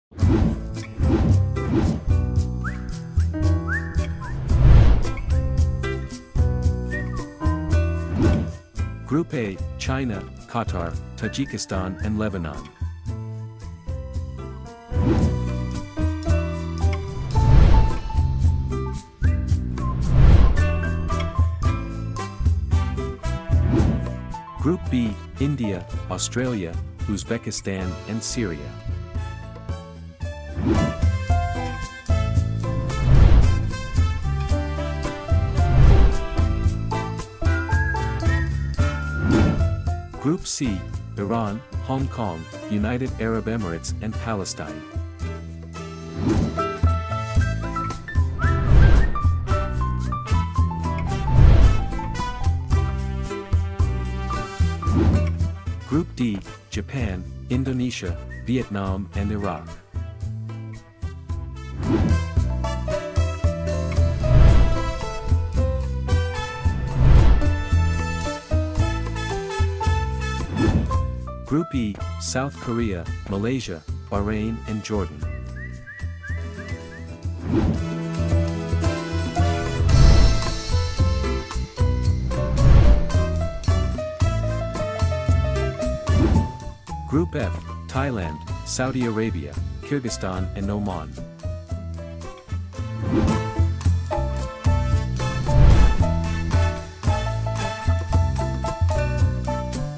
The group table of the 2023 AFC Asian Cup teams -- Countryballs Animation
Hello, I hope you are well, which team do you think will be the champion of the 2023 Asian Nations Cup 😀??